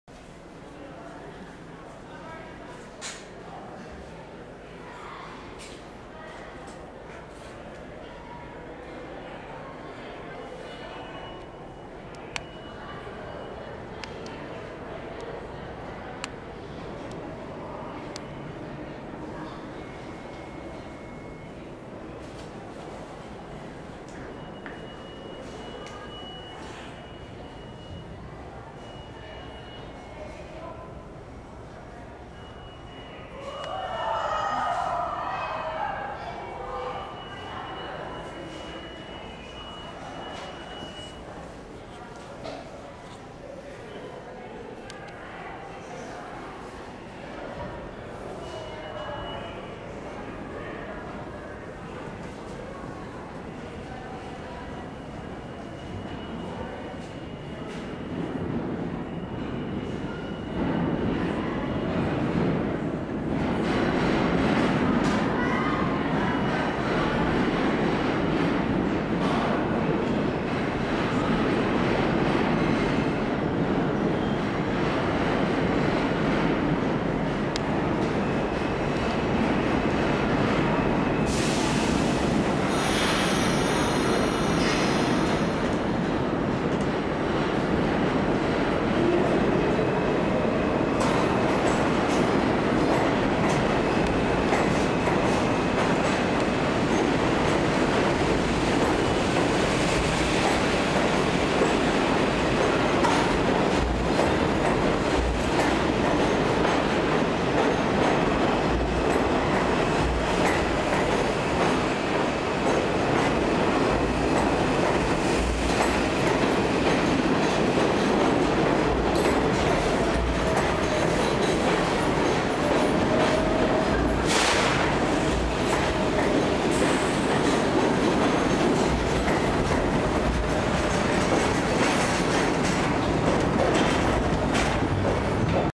Not in Service R46 Arriving and Leave Chambers Street (Bypass) (10:20PM)
taken 3/5/14 at 10:20PM